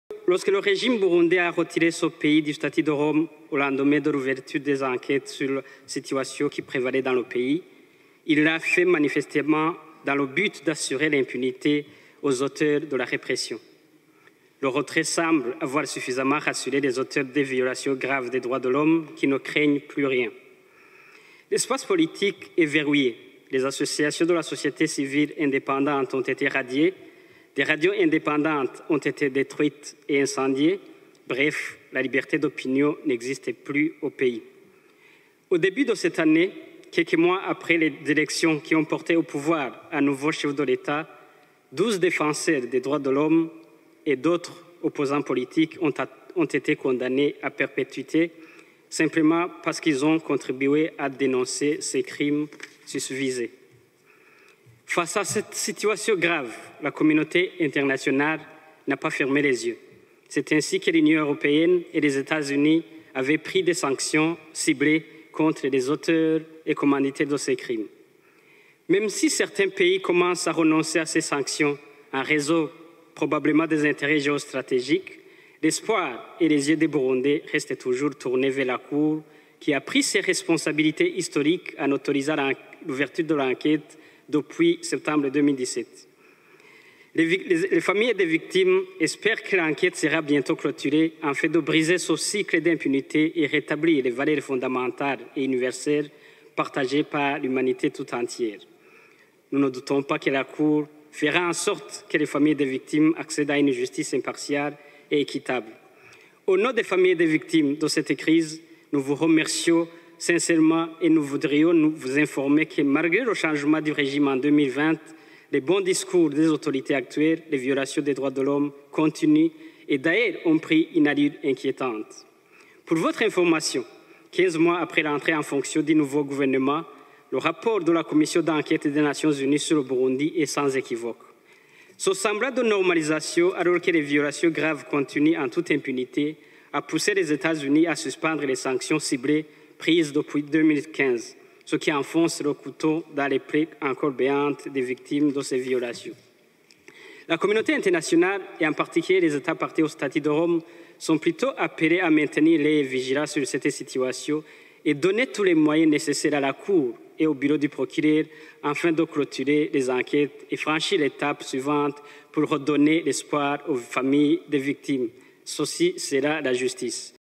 Les victimes de la crise burundaise ont besoin de la justice internationale
Malgré le changement du pouvoir en 2020 et les beaux discours des dirigeants actuels, les violations des droits de l’homme ont continué au Burundi. C’est ce qu’a indiqué dans son discours le président de la Coalition burundaise pour la Cour pénale internationale. Il s’exprimait ce mardi devant l’assemblée des Etats signataires du statut de Rome qui a institué la CPI. Me Lambert Nigarura, président de la CB-CPI, en a profité pour appeler ces Etats à soutenir le procureur de la CPI afin de clôturer les enquêtes et rendre justice aux victimes de la crise burundaise de 2015. Ce discours a été prononcé 4 ans après le retrait du Burundi du statut de Rome.